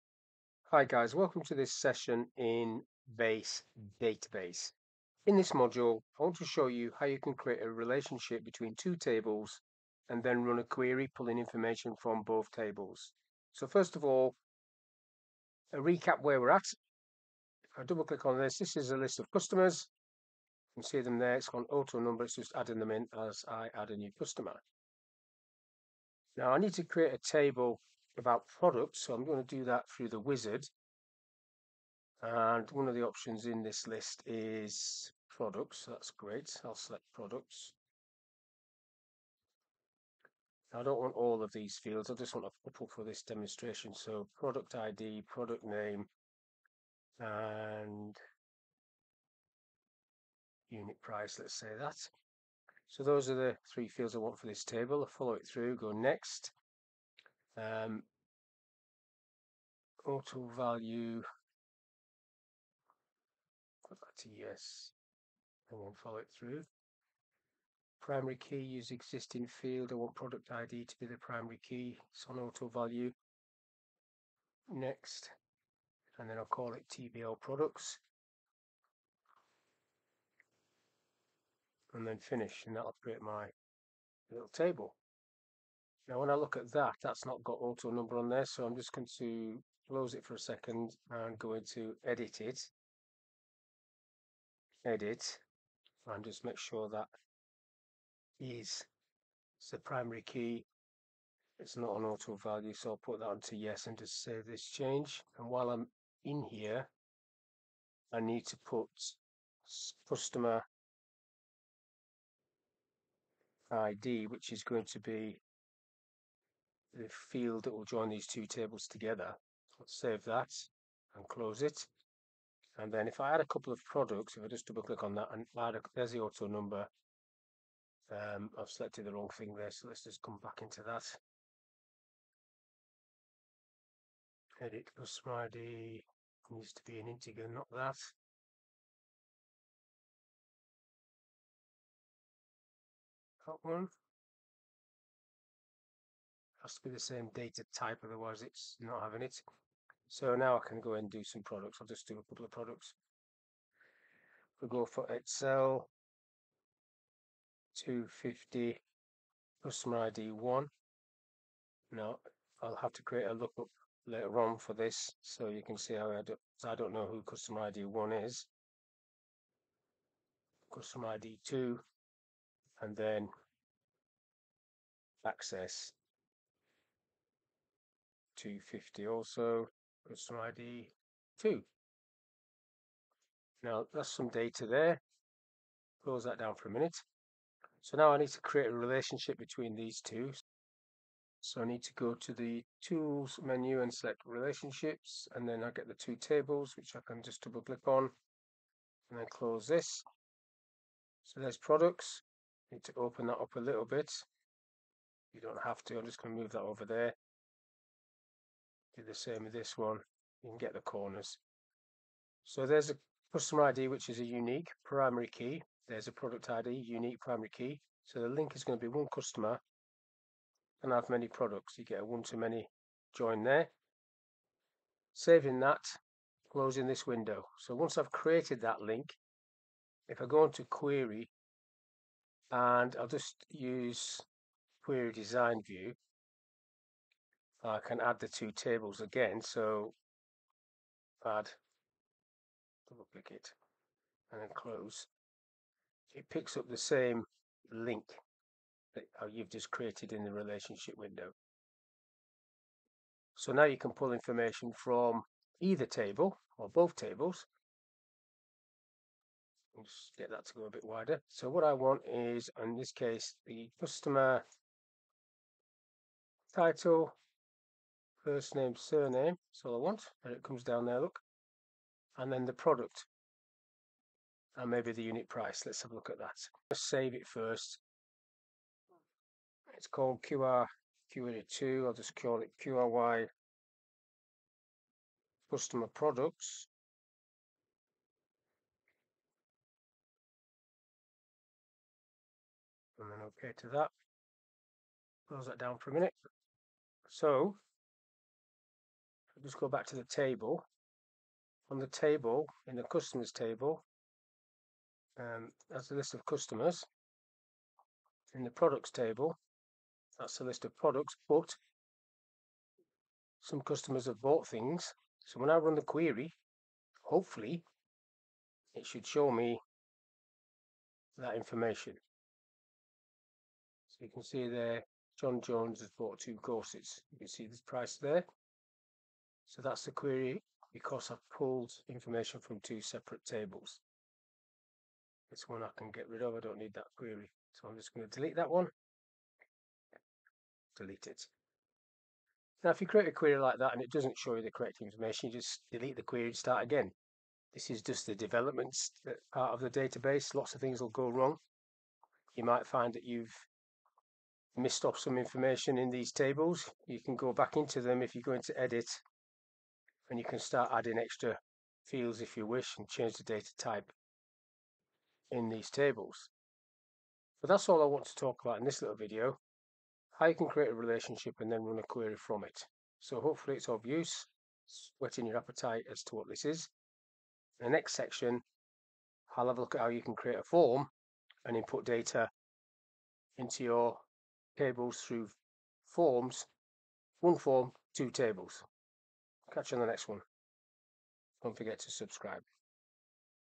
How to Create a Relationship in LibreOffice Base Database!
Watch me show you the easiest way to build a strong relationship in your LibreOffice Base database!
Learn how to create a relationship in LibreOffice Base Database with this step-by-step tutorial. Establishing relationships between tables is crucial for organizing data efficiently and ensuring data integrity. Follow along as we guide you through the process of creating relationships in LibreOffice Base Database, from defining the fields to setting up the desired connections. Enhance your database management skills today!